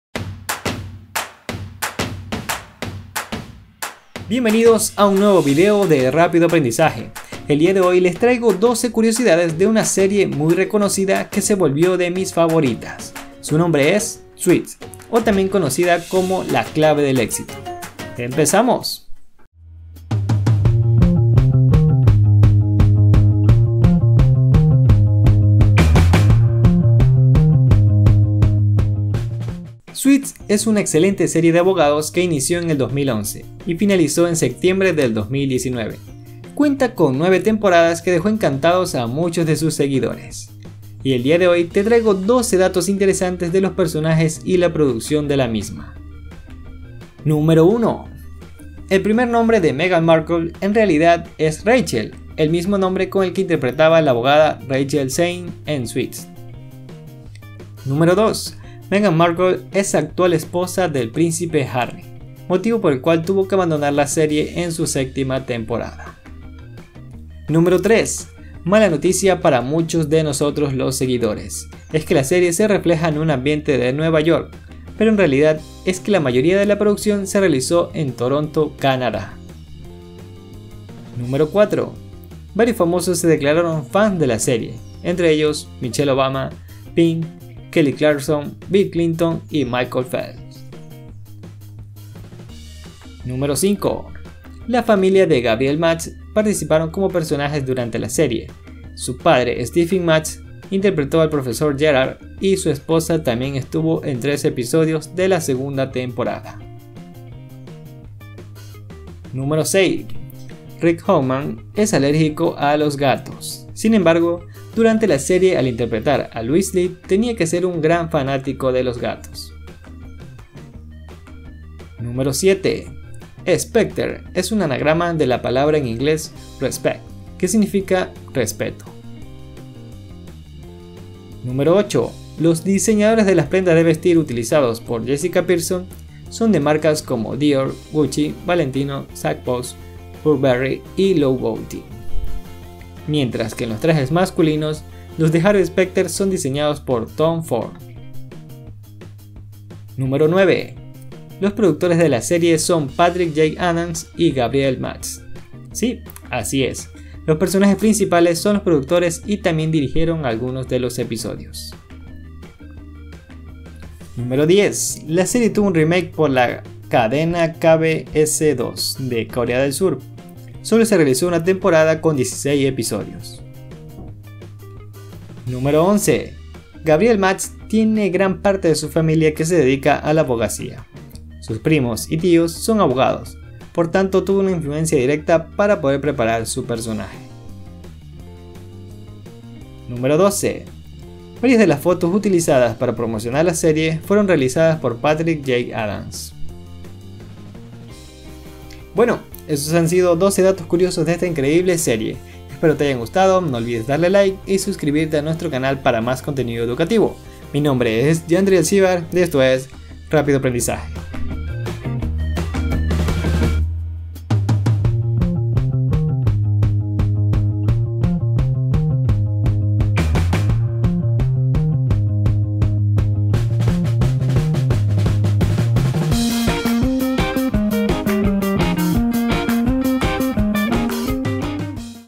12 Curiosidades de Suits / La Ley de los Audaces
Bienvenidos a un nuevo vídeo de Rápido Aprendizaje, el día de hoy les traigo 12 curiosidades de una serie muy reconocida y que se volvió de mis favoritas, su nombre es “Suits” o también conocida como “La clave del éxito”.

🔊Escucha nuestro PODCAST en:
_ Apple Podcast, Spotify, Google Podcast, Ivoox, etc.